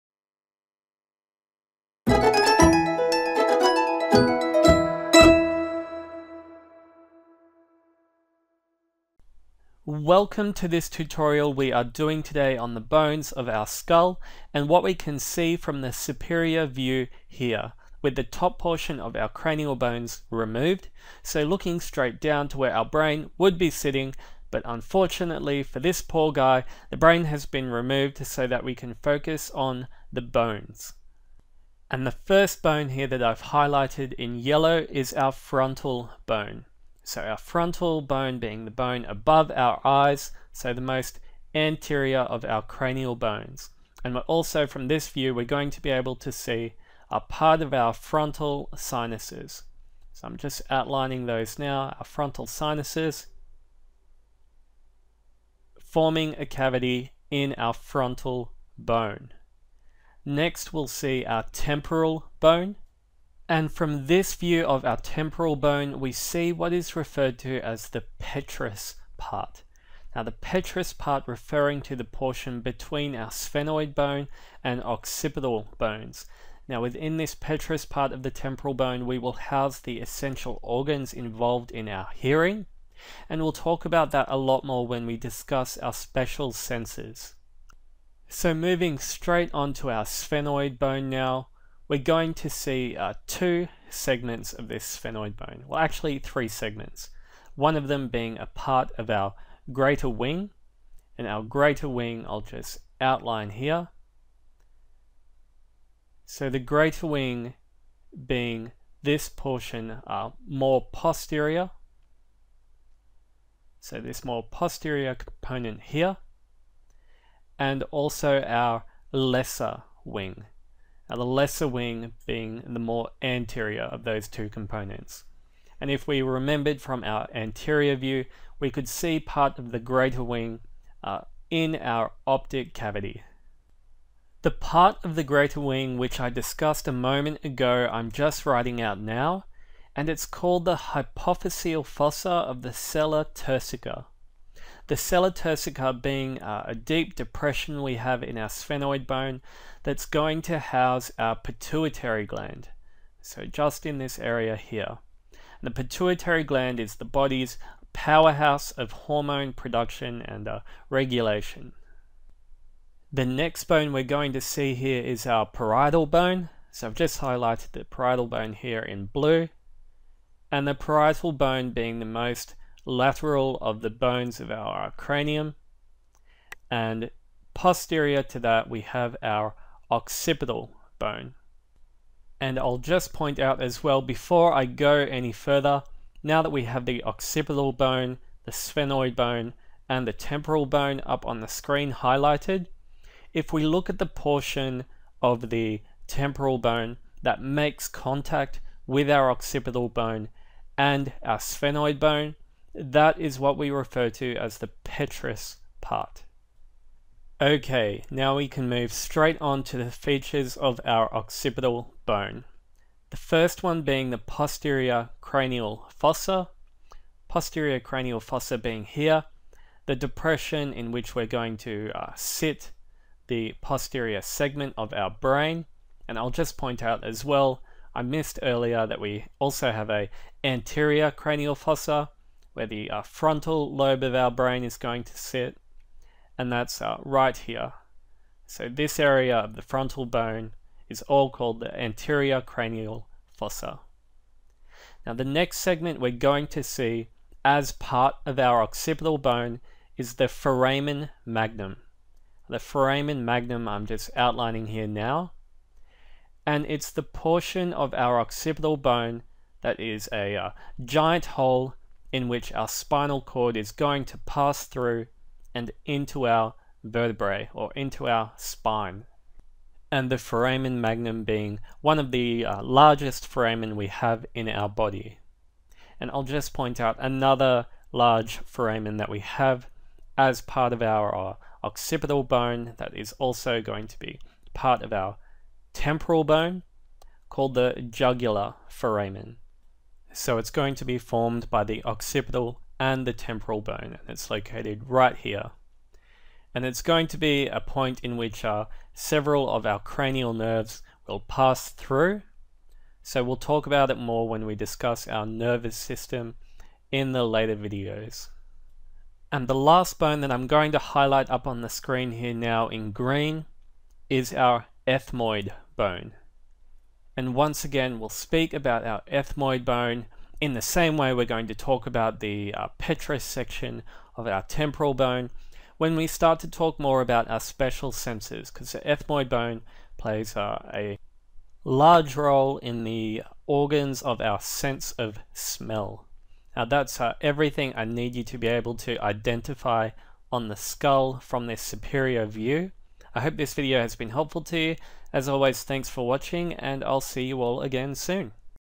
Bones of the Skull- Superior
Focussing on a superior aspect, this tutorial takes a look into our skull and helps us identify each structure that we can see from this view.

Video number- Anatomy 1- 43